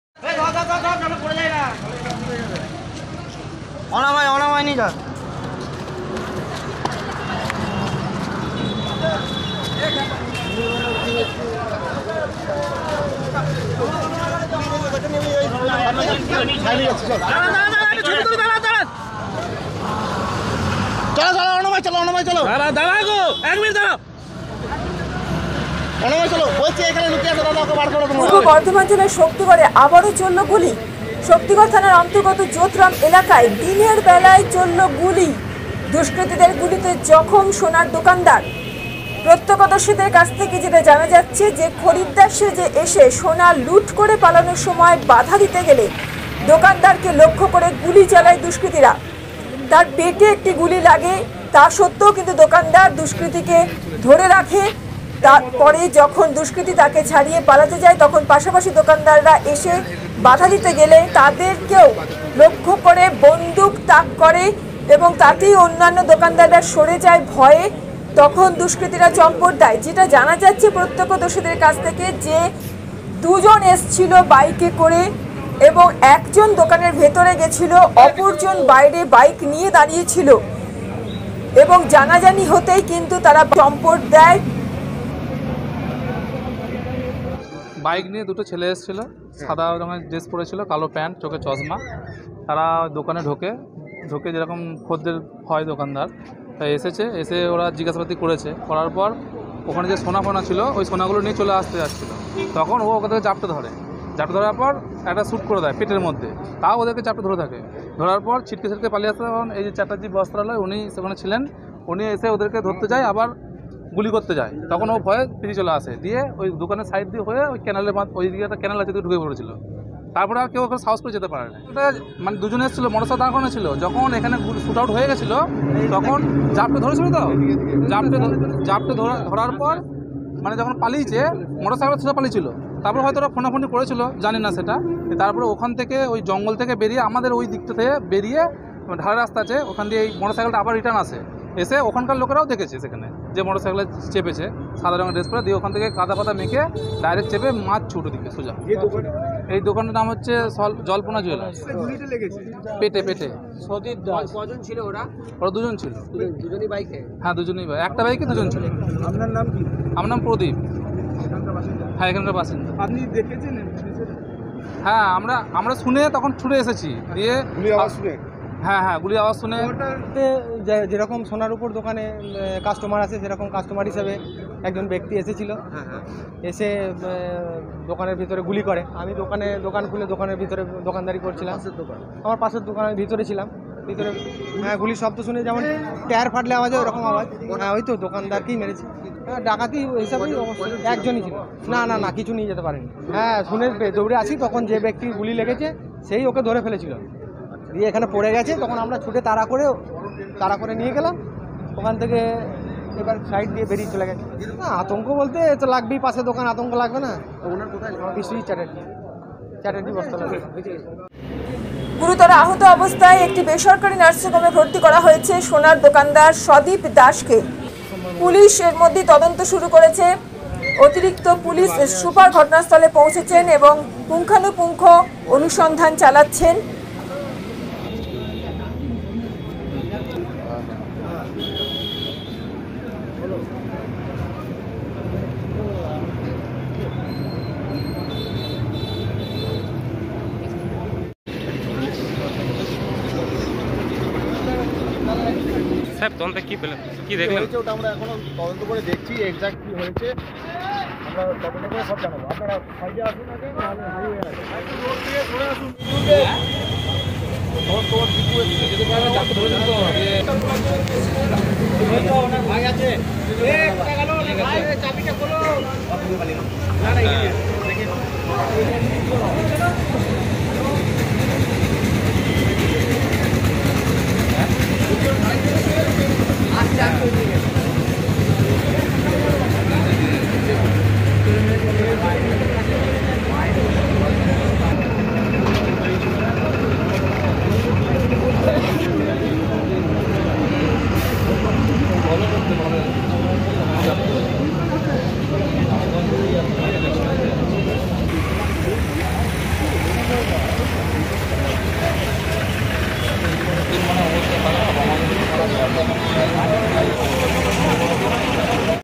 শক্তিগড় থানা এলাকায় আবার চলল গুলি
পূর্ব বর্ধমান জেলার শক্তিগড় থানার অন্তর্গত  জোতরাম এলাকায় দিনের বেলায় চললো গুলি ।  দুষ্কৃতীদের গুলিতে জখম সোনার দোকানদার । খরিদ্দার সেজে এসে সোনা লুট করে পালানোর সময় বাধা দিলে দুষ্কৃতীরা গুলি চালায় দোকানদারকে লক্ষ্য করে। পাশাপাশি দোকানদাররা বাধা দিতে এলে তাদেরকেও গুলি করতে যায় দুষ্কৃতীরা । আতঙ্কে সরে যায় পাশাপাশি দোকানদাররা । তখন চম্পট দেয় দুষ্কৃতীরা। জানা গেছে বাইক নিয়ে এসেছিল দুই দুষ্কৃতি। গুরুতর আহত অবস্থায় একটি বেসরকারি নার্সিংহোমে ভর্তি করা হয়েছে সোনার দোকানদার সদীপ দাস কে। পুলিশ তদন্ত শুরু করেছে।